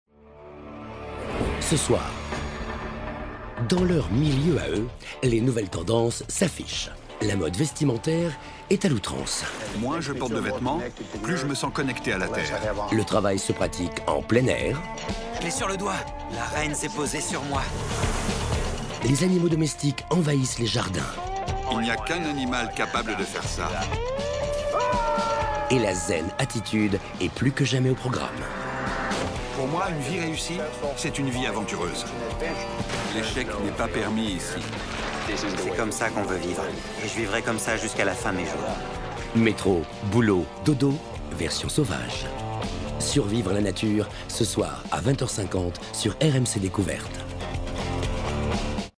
Survivre à la nature : les ours et les abeilles
Des aventuriers ont décidé de vivre en plein accord avec la nature. En Caroline du Nord, un couple essaie différentes méthodes afin d´attirer des essaims d´abeilles dans leurs ruches.